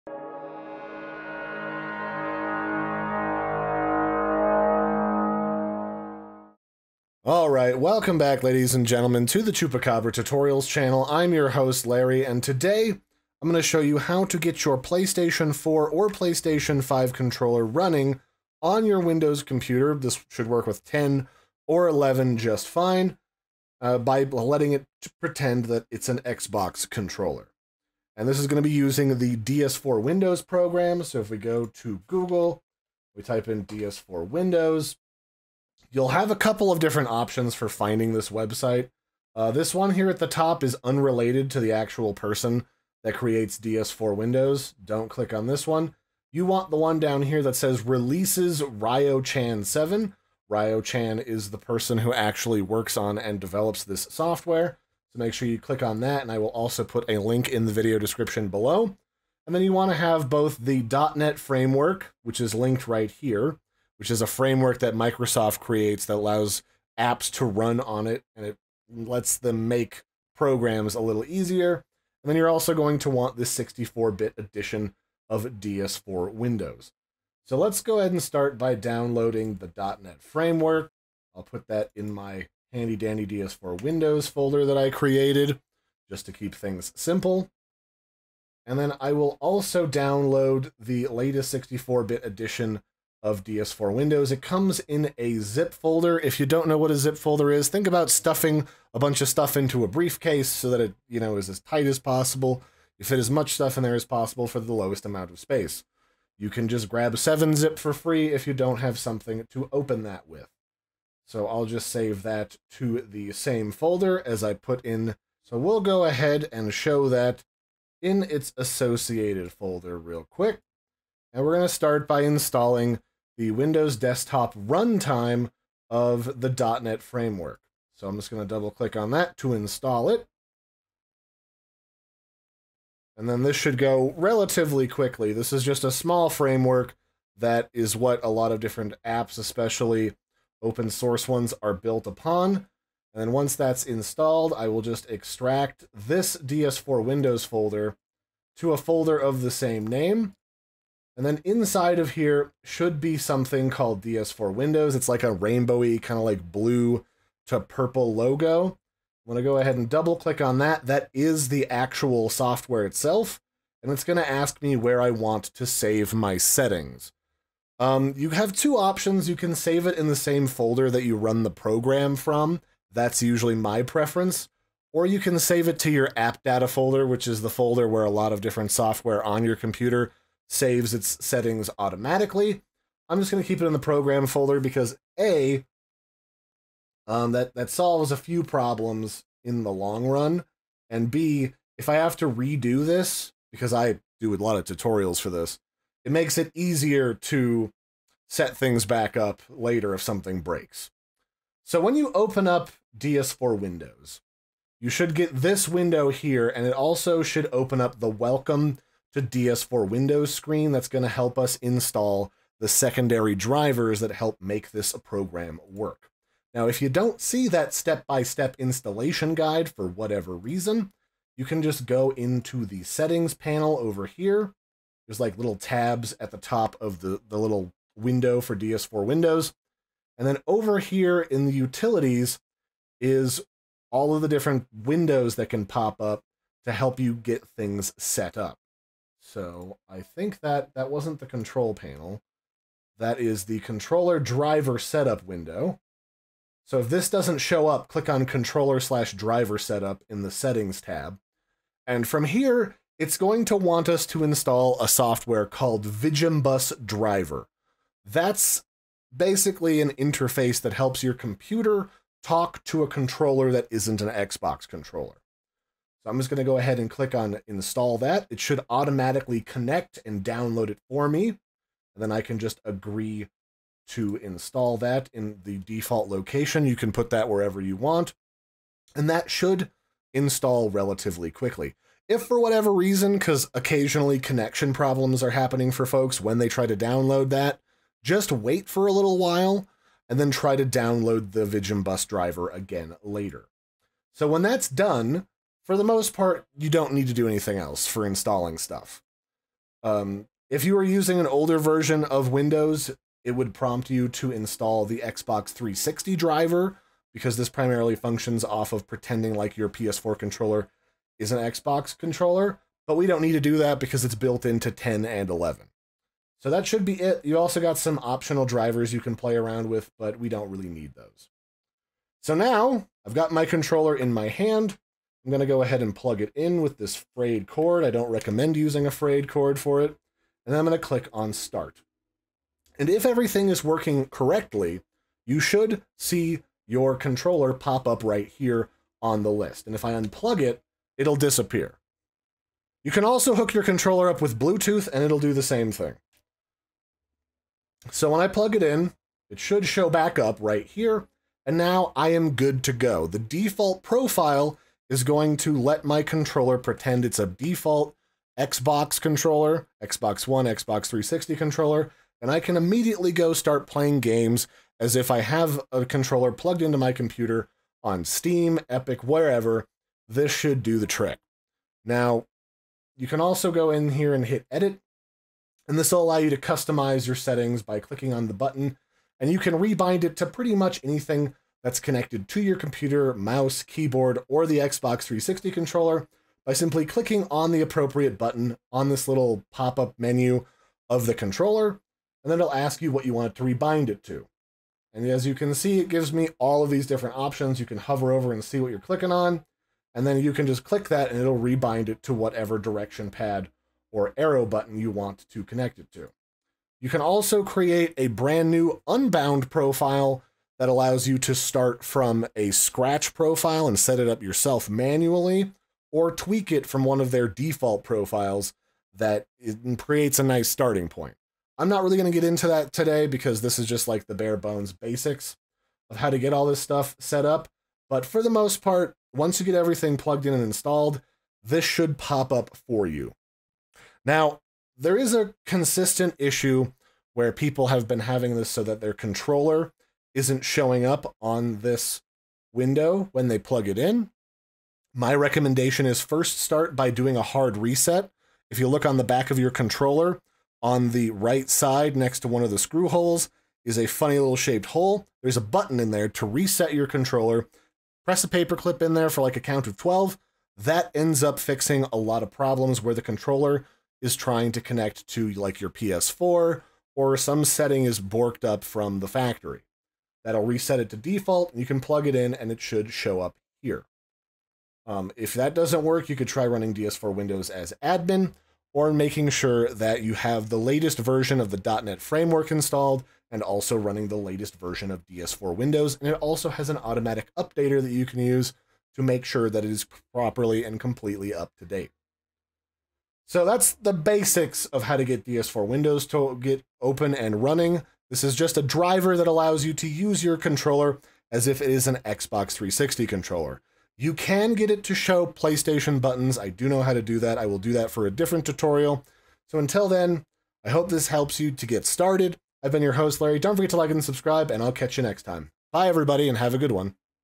2023: How to Connect PS4 Controller to PC with DS4 Windows - Updated
Complete guide to setting up your PS4 & PS5 controller on your windows pc using the DS4 Windows Driver. Just connect your controller with cable or with bluetooth, then install the .NET Framework which allows DS4 to run. Once complete you just need to run the DS4 app from its unpacked folder, download any remaining drivers your computer needs, and hit start to begin using your controller for all your games.

This guide walks you through downloading and installing all required software and settings.